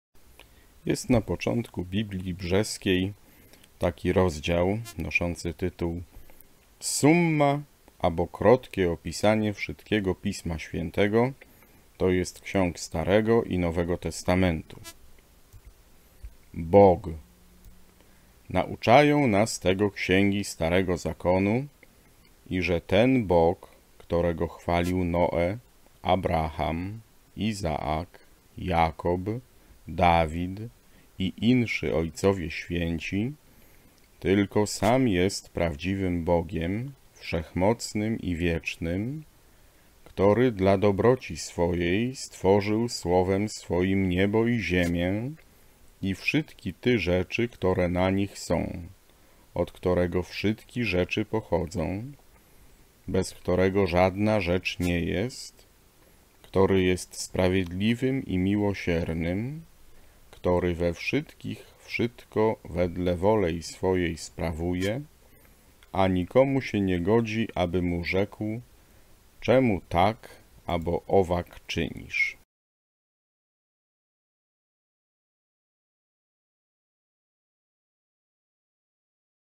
Brzeska Summa 01
Czytamy XVI-wieczne Biblie. Język ich ciekawy, nieraz zadziwiający.